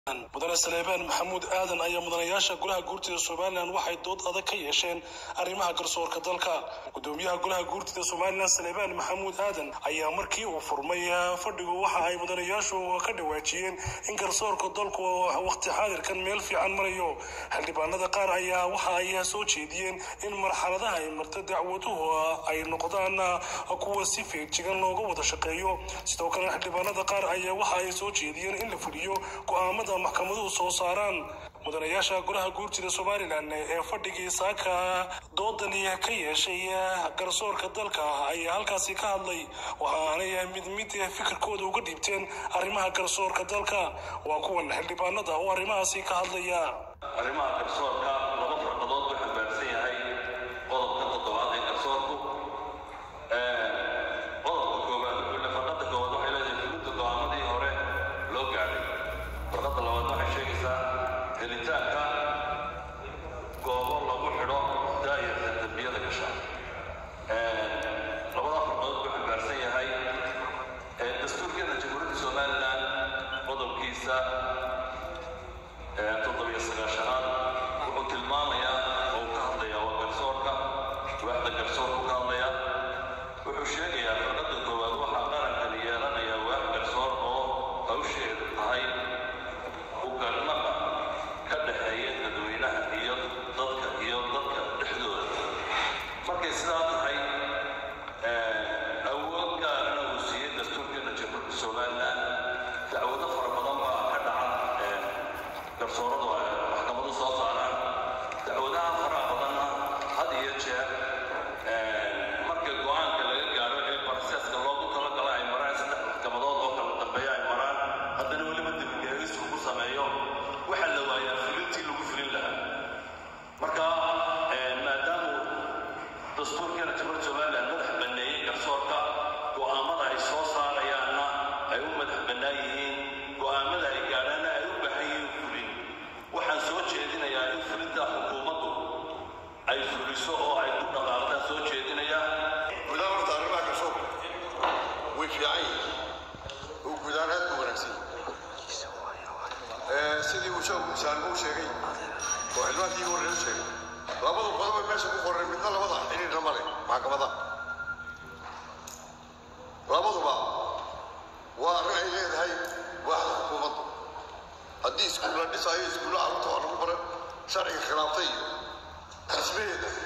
Golaha Gurtida JSL o0 Dood Adag Ka Yesheen Arrimaha Garsoorka Dalka
Hard Talk on Simaliland House of Lords